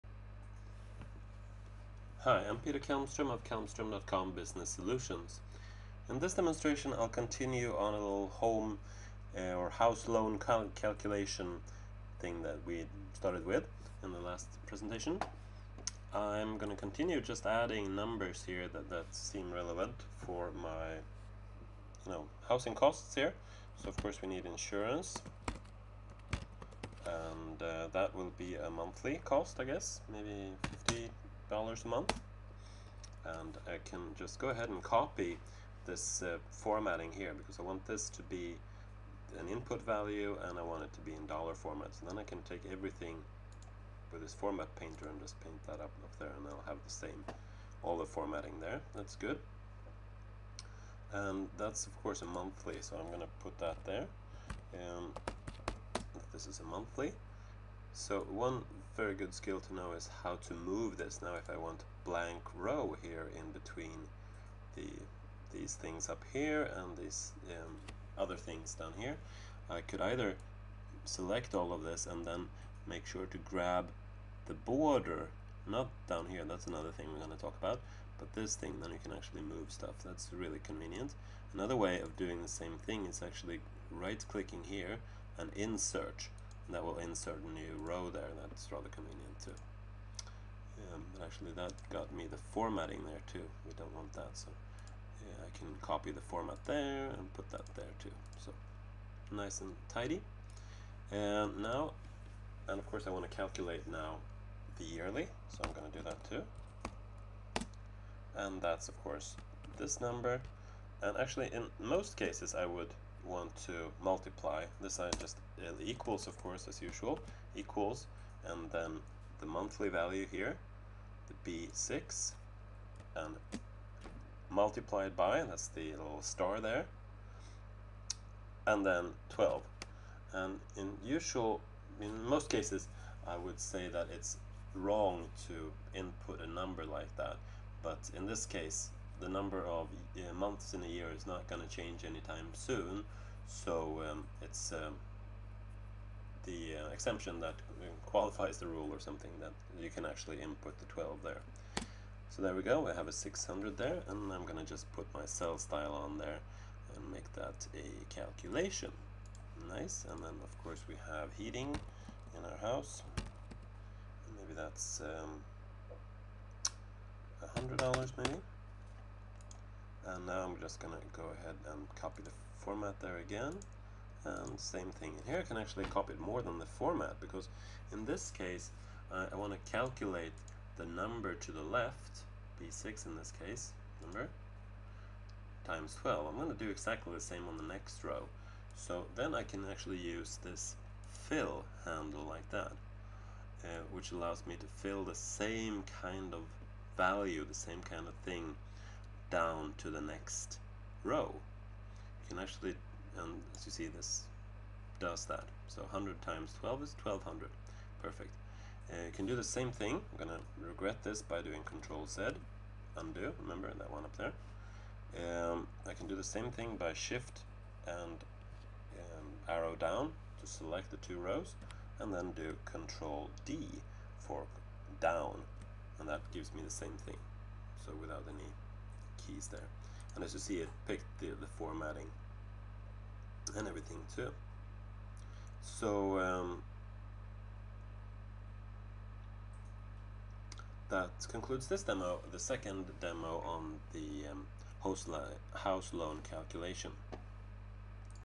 Excel 2016 from Scratch.8 - House Costs Calculation B
Excel 2016 beginners course, also suitable for earlier versions. This is the second demo on how to use formulas to calculate housing costs.